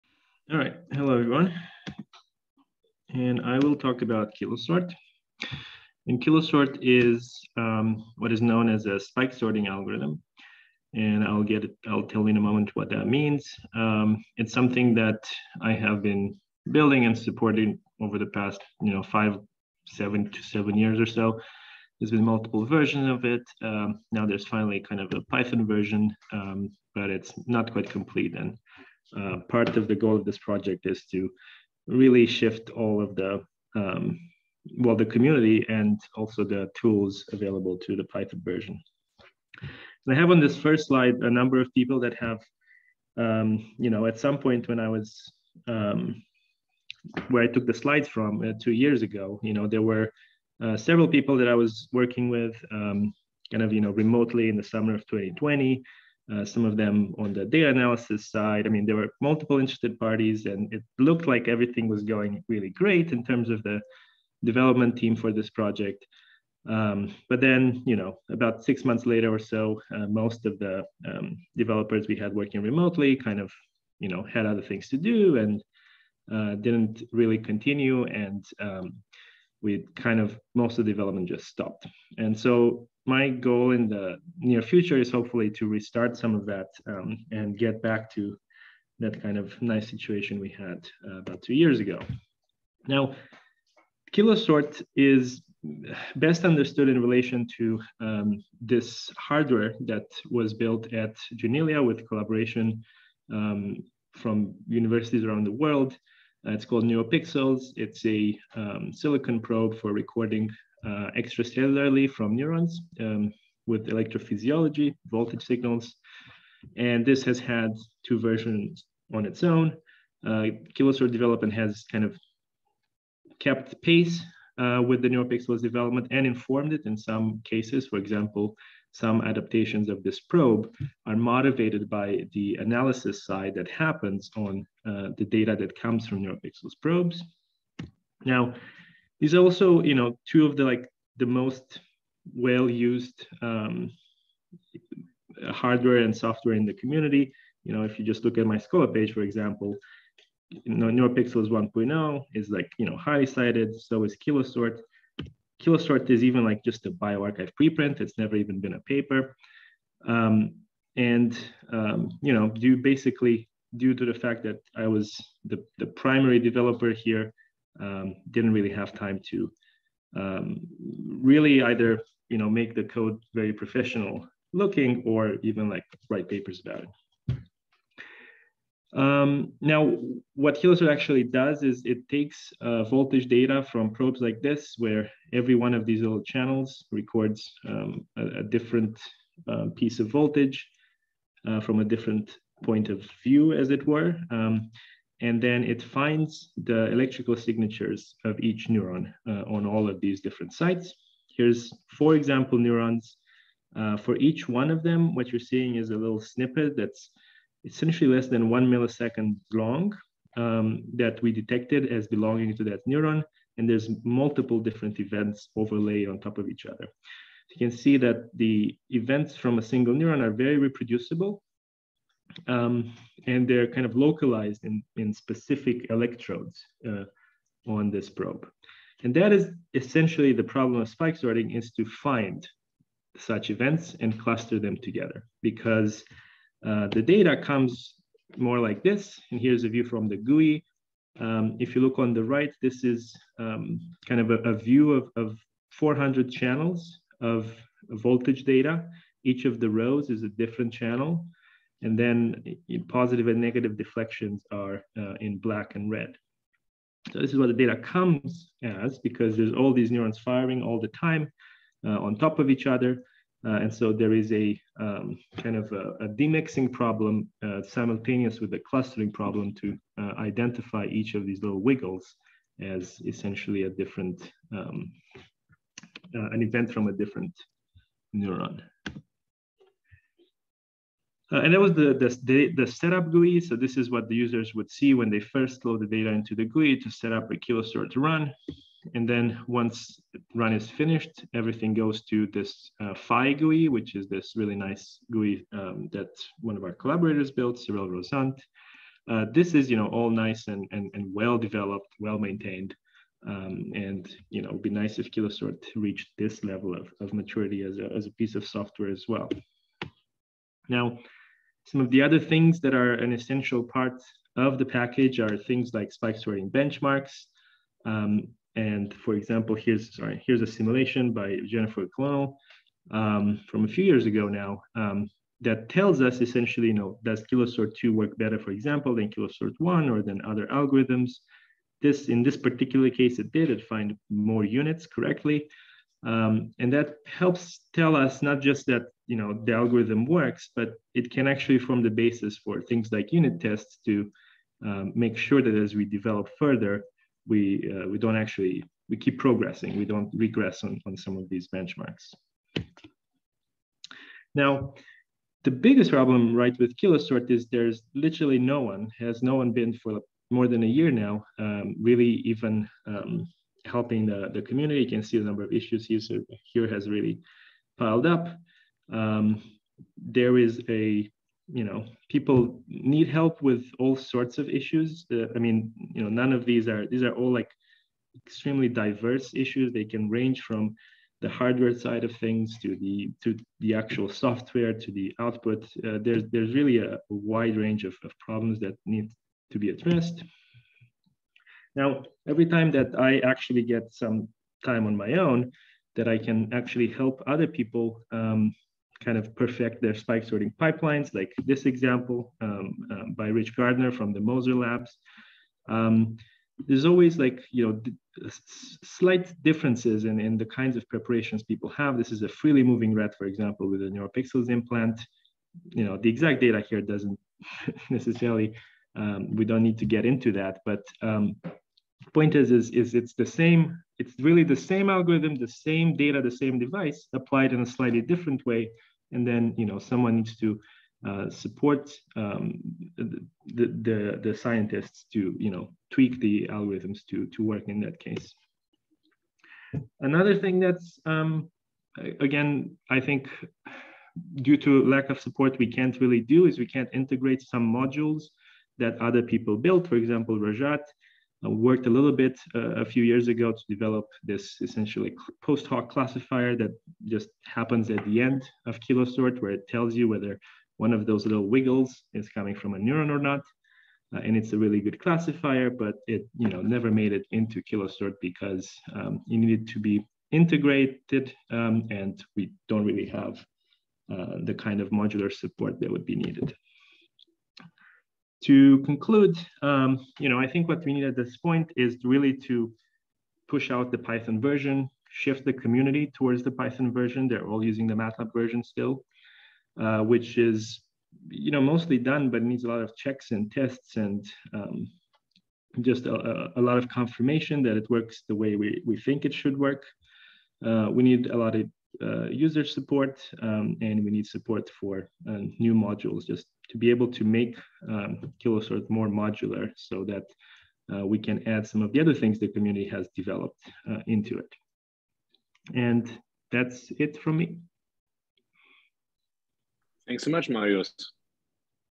KiloSort
A description of KiloSort by Marius Pachitariu, which is supported by the Open Science Software Initiative. and on GitHub